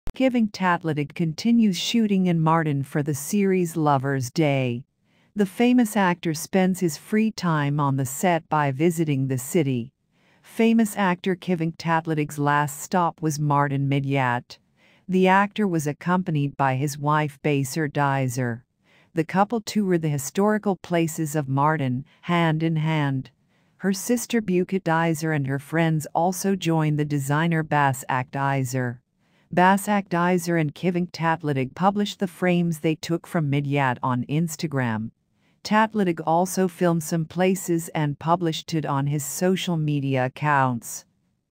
Başak Dizer and Kıvanç Tatlıtuğ romantic historical trip!
Kivanc Tatlitug continues shooting in Mardin for the series ' Lovers ' Day '. The famous actor spends his free time on the set by visiting the city. Famous actor Kivanc Tatlitug's last stop was Mardin Midyat. The actor was accompanied by his wife Basar Dizer. The couple toured the historical places of Mardin, hand in hand. Her sister Buket Dizer and her friends also joined the designer Basak Dizer. Basak Dizer and Kivanc Tatlitug published the frames they took from Midyat on Instagram. Tatlitug also filmed some places and published it on his social media accounts.